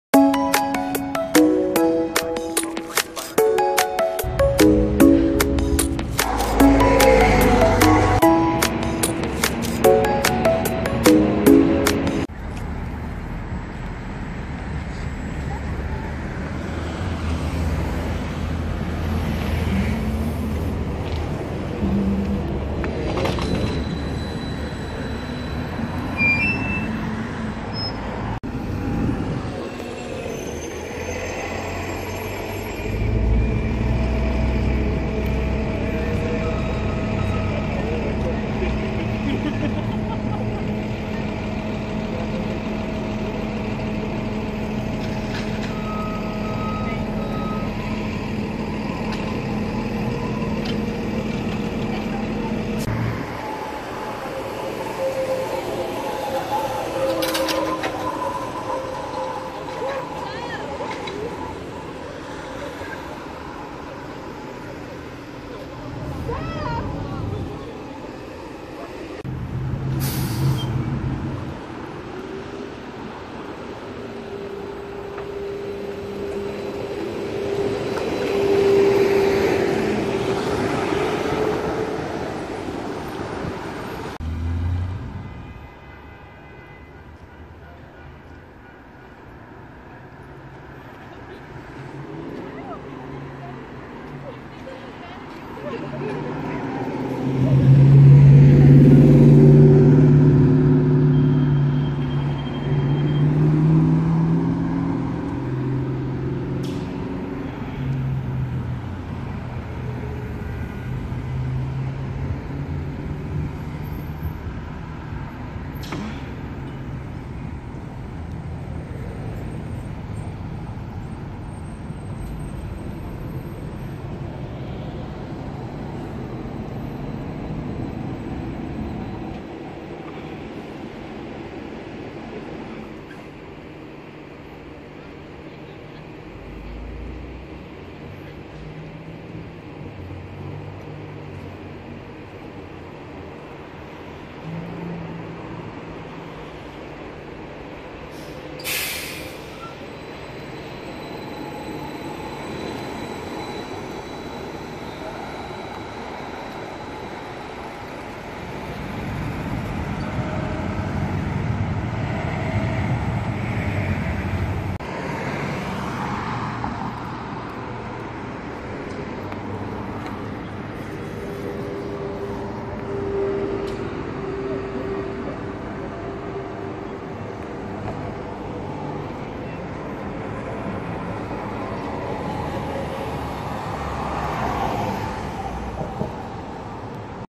Trolley buses around Vancouver Part 2 ( Read Description for info)
Welcome to another video of this series of trolley buses in Vancouver.

Here's the list:

0:12 / 2534 is seen crossing E 41st heading south towards it's end on Marine Drive

0:29 / 2285 is seen pulling into Metrotown with some up close switch actions

0:53 / 2185 is seen going through a switch really up close across Fraser Street

1:09 / 2534 is seen again departing E 41st heading south

1:25 / 2531 is seen not far from E 41st with its poles down after going to fast over a switch at Fraser street @ E 41st, now the driver comes out to put the poles back, while the driver is putting the poles 2517 passes north bound for downtown while 2559 pulling behind 2531 as 2531 is late

2:45 / 2543 is seen Dead heading to the depot along W 41st while 2182  follows behind.